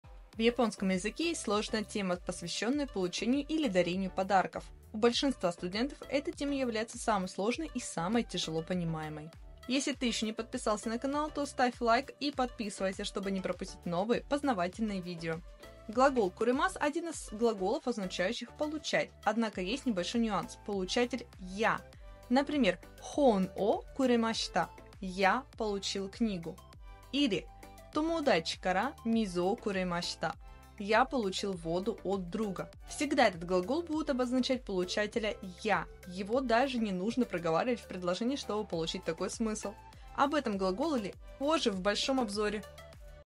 Глагол くれます в японском языке!
Что вы знаете о глаголе くれます в японском языке?
Это глагол означает получать какую то вещь или получать помощь!
Подробнее скоро в большом обзоре на 3 знаменитых глагола!